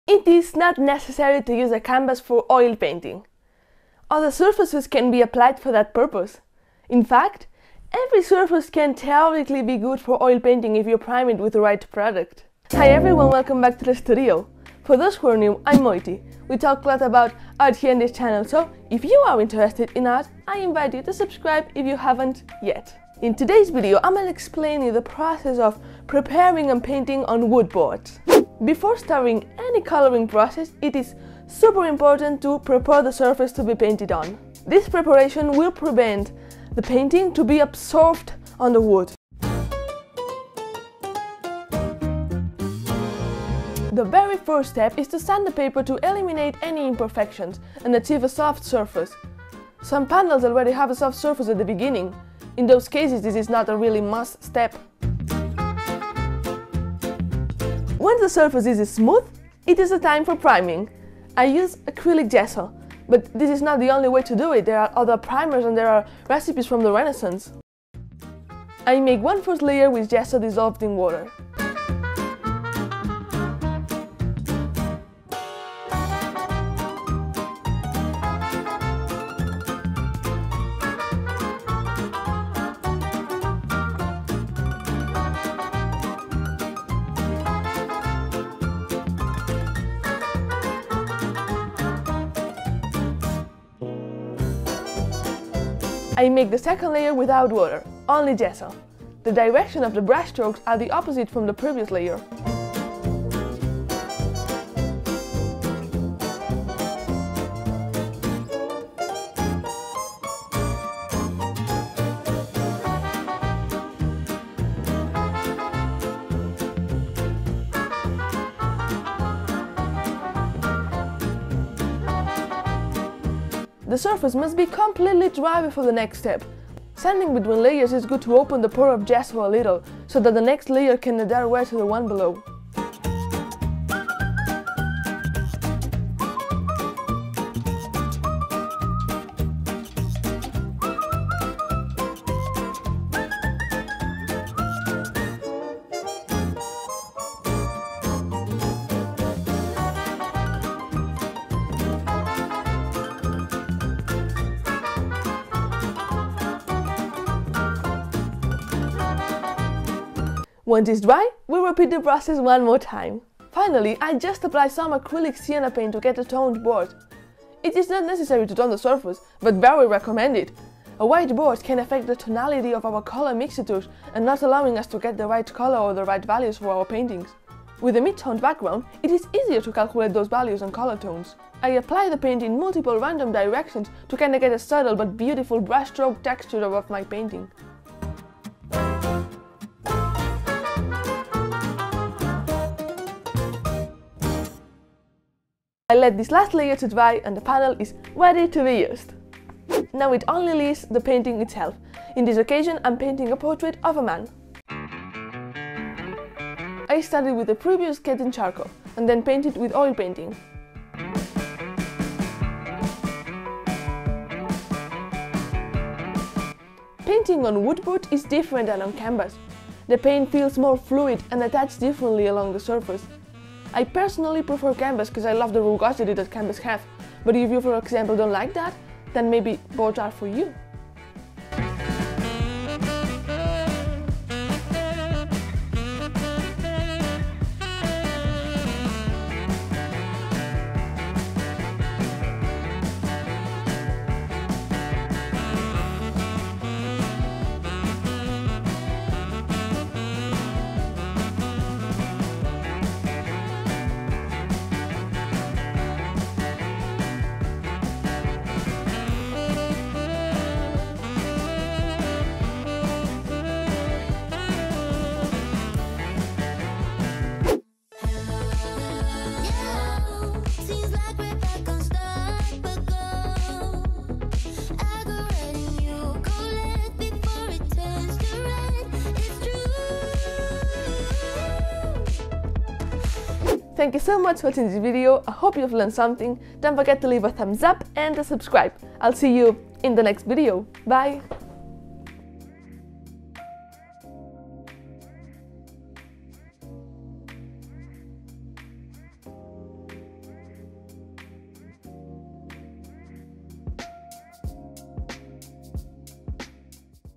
How To Paint On Wood Panels with Oil Painting
I explain you how to prepare / prime  and paint on a wood panel with oil painting.

Esther Ebale